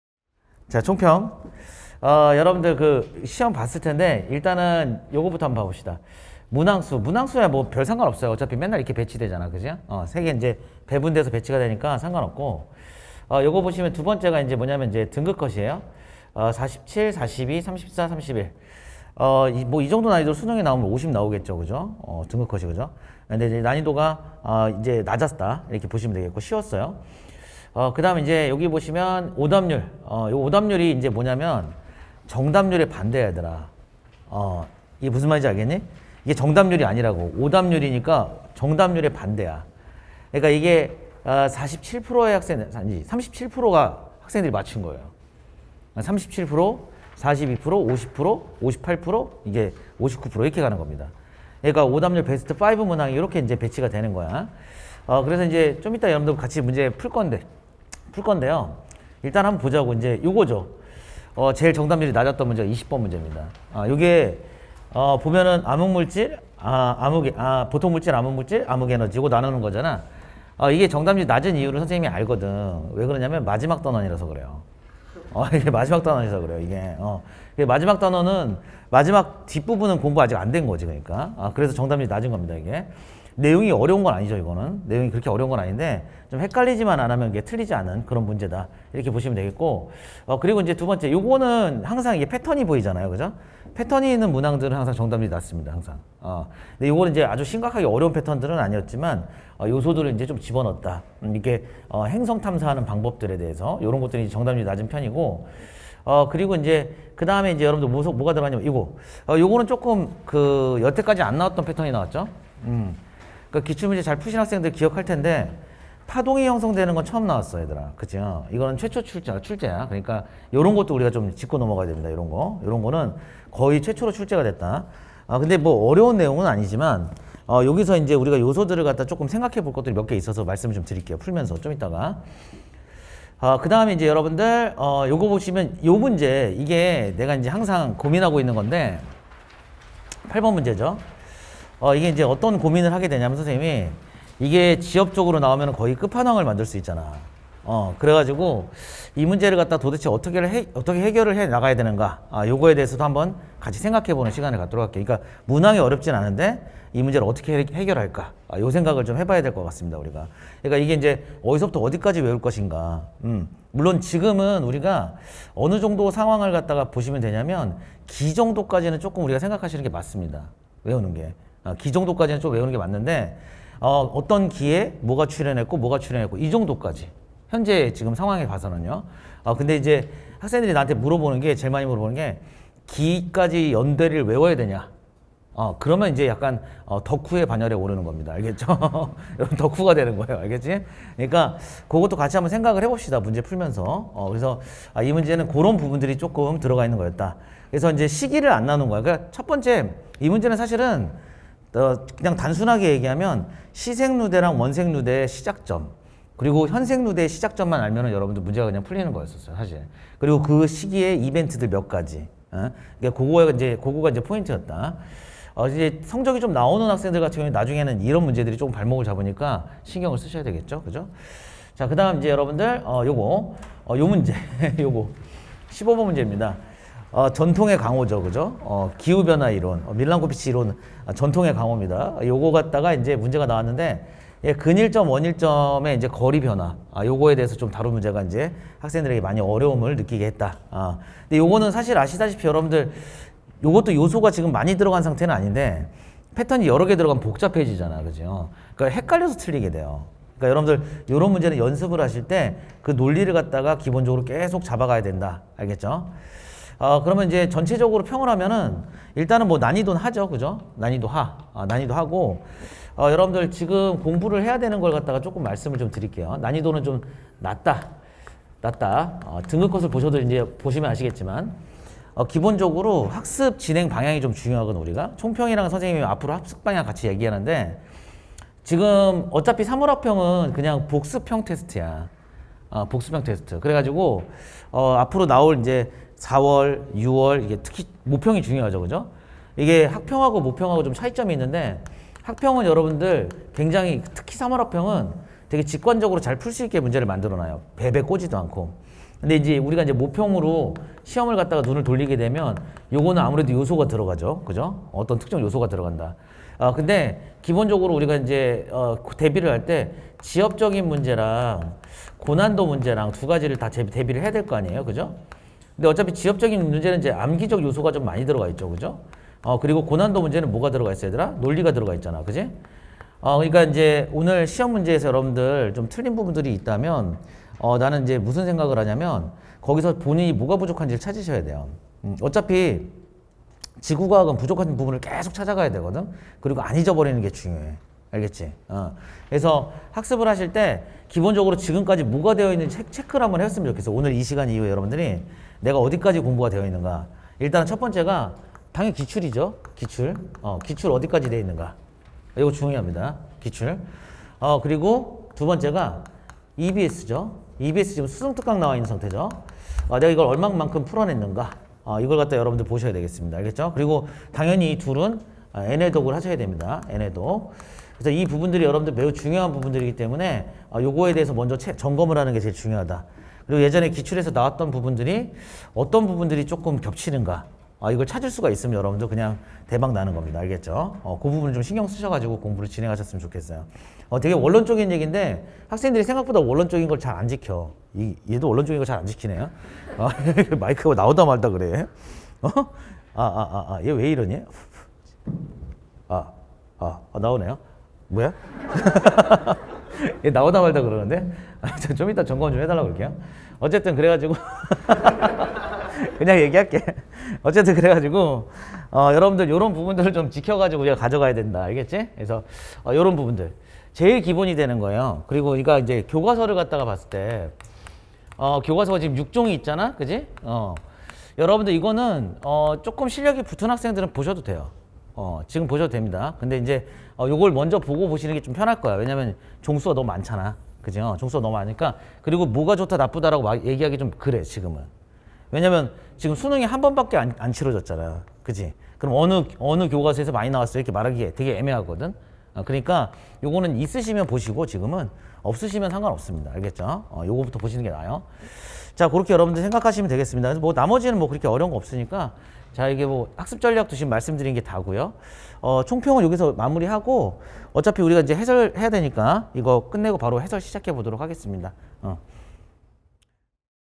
[메가스터디] 지구과학 박선쌤 - 고3 3월 학력평가 지구과학l 총평
3월 학평 보느라 고생 많으셨습니다.
난이도와 관계없이 틀린 문제를 통해 본인의 약점을 찾고,
이를 보완하는 앞으로의 학습이 필요합니다.

박선 선생님 2022학년도 3월 학평 총평을 영상으로 확인하세요